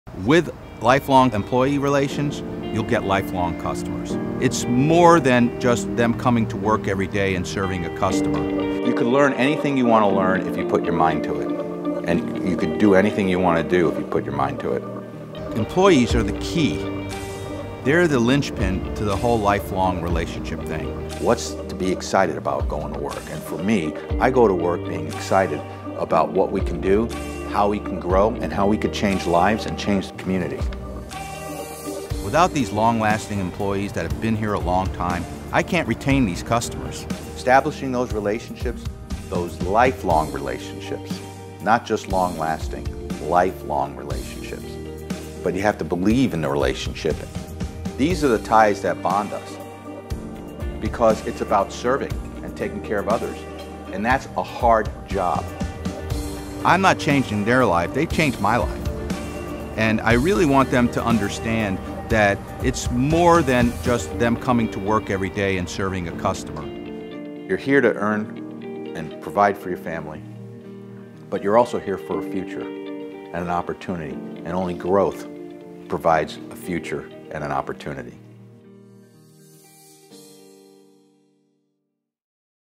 Cool, Plumbing & Energy Today Lifelong Relationships
Cooltoday.com

Cool, Plumbing and Energy Today Lifelong Relationships are formed everyday within our team and with our customers. With Lifelong employee relations, you will get lifelong customers. It’s more than them just coming to work everyday and serving the customers. You can learn anything you want to learn and you can do anything you want to do as long as you put your mind to it. Employees are key. They are the ones who strengthen all of our lifelong relationships. We go to work excited. We can’t wait to see what we can do, find out how we can grow and and learn how we can change lives and our community. Without our long lasting employees, we wouldn’t be able to retain our customers and establish those lifelong relationships.

Cool, Plumbing and Energy Today Lifelong Relationships are founded on the idea that you have to believe in those relationships in order for them to last. We trust in our customers and our team members everyday, whether we are working or meeting their families, because that is what bonds us. It’s about serving. It’s about taking care of others. That’s a hard job. We aren’t changing our customers and employees lives, they are changing ours. It’s more than coming to work everyday and serving a customer. It’s not a daily task list. Our employees are here to earn and provide for themselves and their families. They are here to create a future and opportunity. They are here to find growth.

Cool, Plumbing and Energy Today Lifelong Relationships are so valuable because we all live in the same community. We shop at the same grocery stores, our kids go to the same schools. We’ve been serving your community, the Southwest Florida community, for over 50 years. And it’s our pleasure to do so. Because if we live in a community, it’s our duty to give back any way we can.

Cooltoday.com